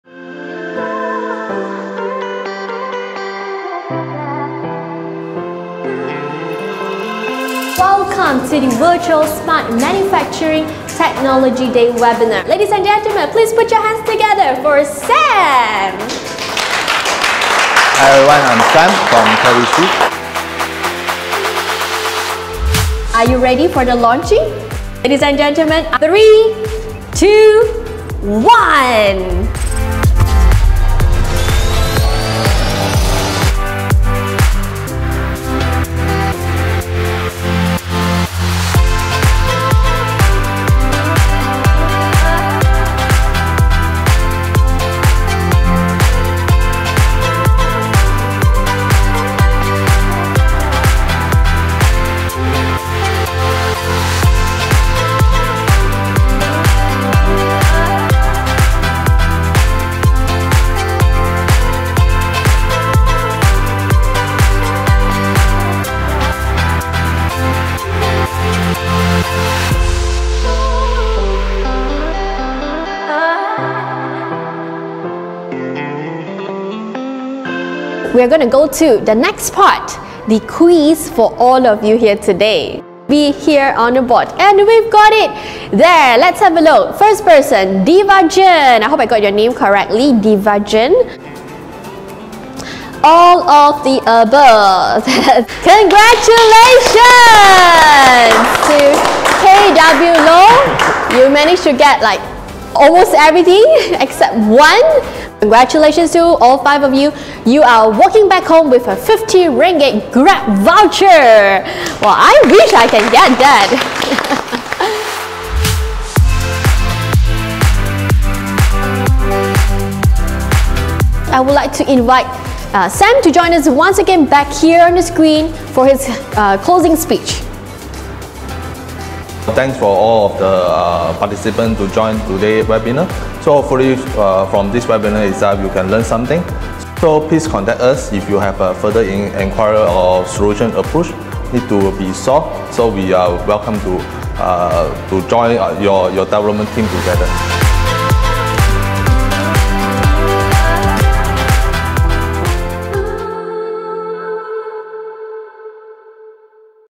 KVC Smart Manufacturing Technology Day
KVC partners with Rockwell Automation, ProSoft, Baumer, and Rittal for the Smart Manufacturing Technology Day via the Zoom platform on 5th July 2022. The event was held with the aim of creating more awareness for Industry 4.0 as the world moves rapidly towards automation.